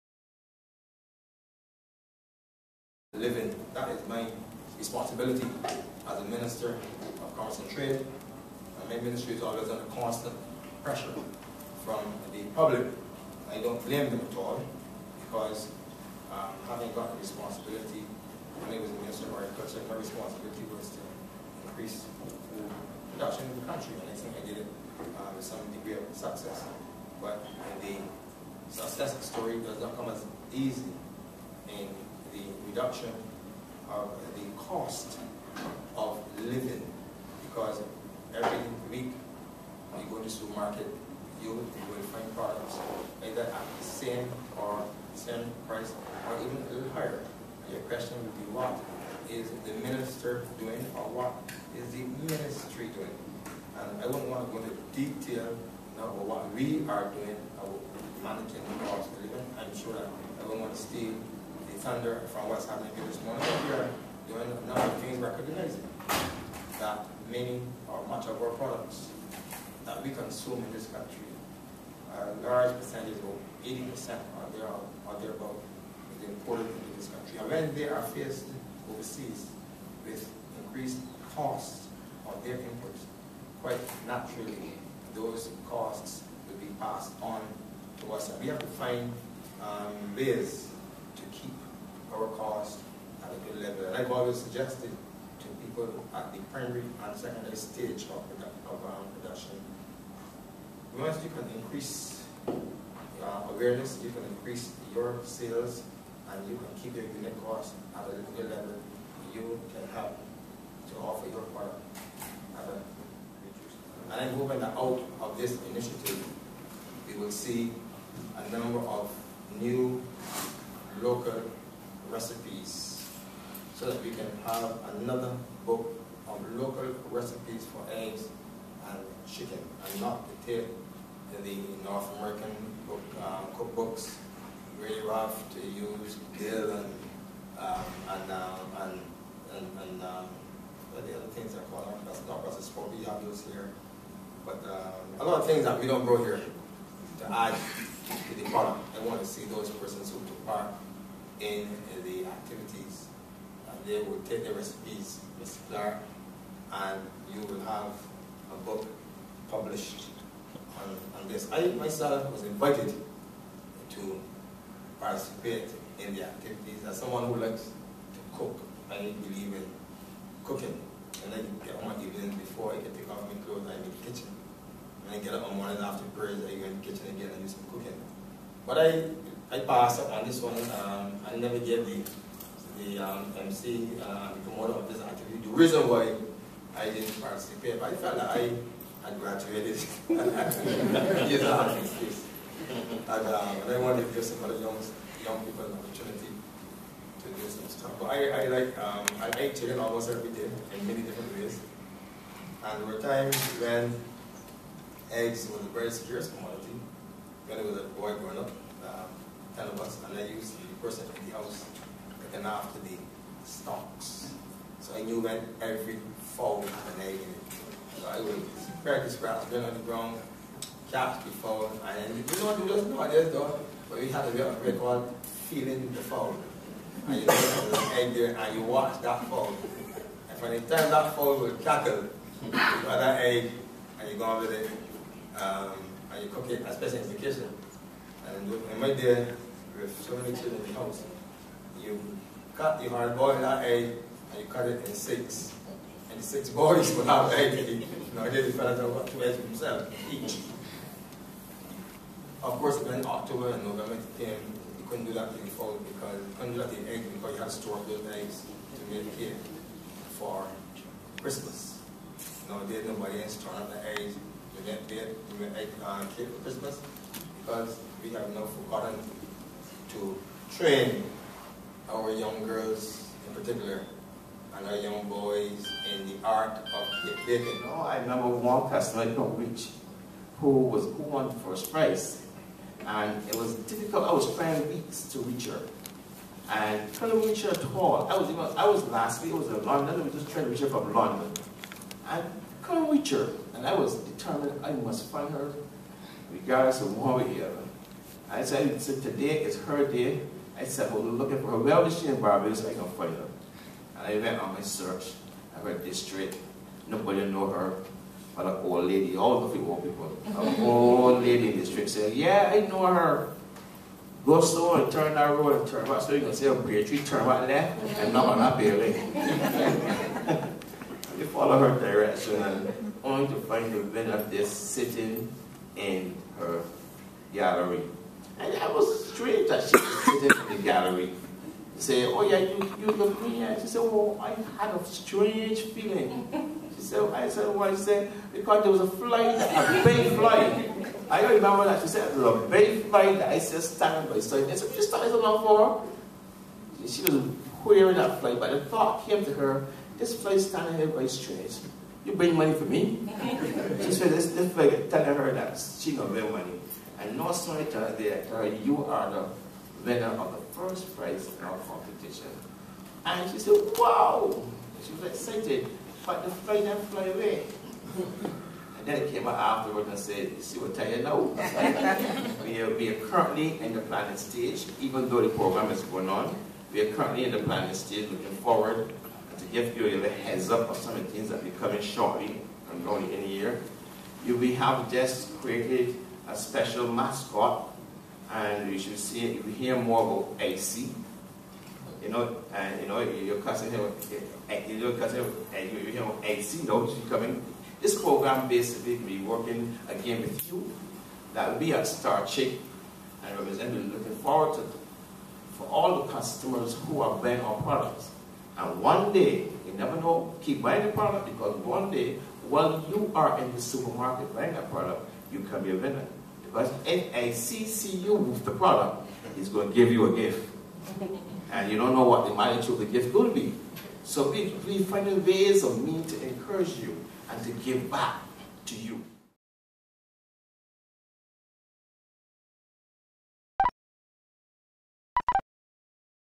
Star Chick Invites Commerce Minister for Prize Ceremony, Sen. Benn avoids Interview
Senator Benn assumed office in 2010, he was asked what about CLICO and the revised Basket Of Goods for the boosted VAT of 17.5% and told Bajan Repiorter "this is early days yet" and needed time to settle in his office... 2012 is nearly over and Barbados is yet to learn what Commerce will do in conjunction with Finance & Econ. Aff's. to reimburse CLICO policyholders who were told by the late PM David Thompson to keep paying your deposits as CLICO is safe? Once the prize giving ceremony was over, the man nearly galloped away as media came Fwd to ask Questions - this despite the fact he claimed in his Address how the Commerce Ministry should be held accountable for Cost Of Living!